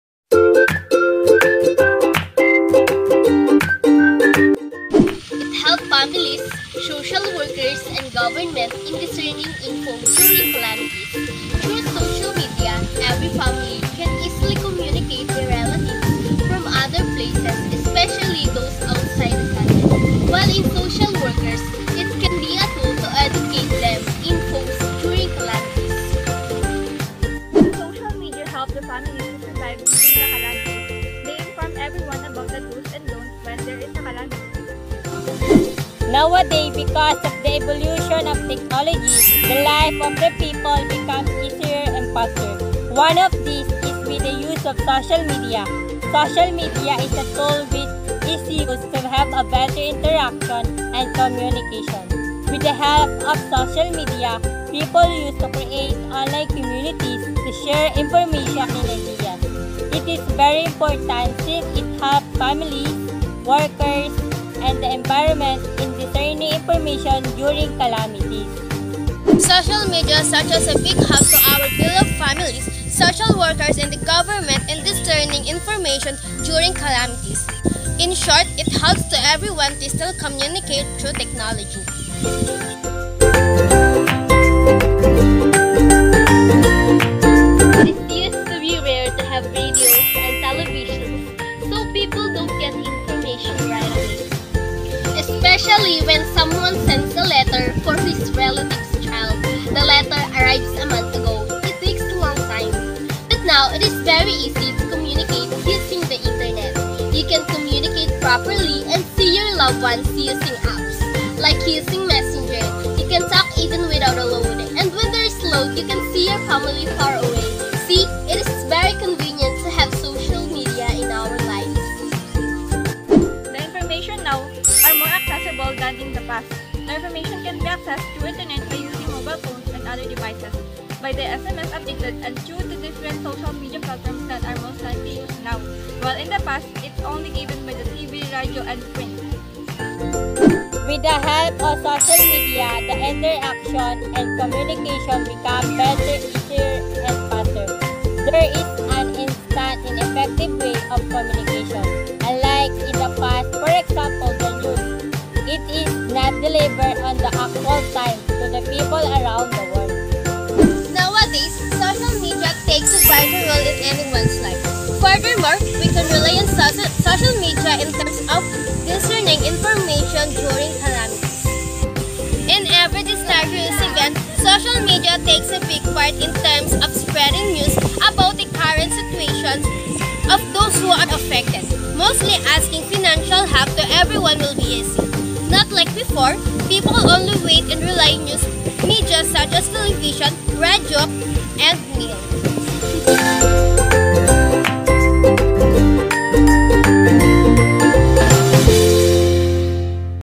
How social media has helped families,social workers,and the goverment during calamities?
HowsocialmediahashelpedfamiliessocialworkersandthegovermentindiscerninginformationduringcalamitiesDifferentiateitfromthepastwhereinformationisonlyprovidedbythemedia (TVradioandprint)?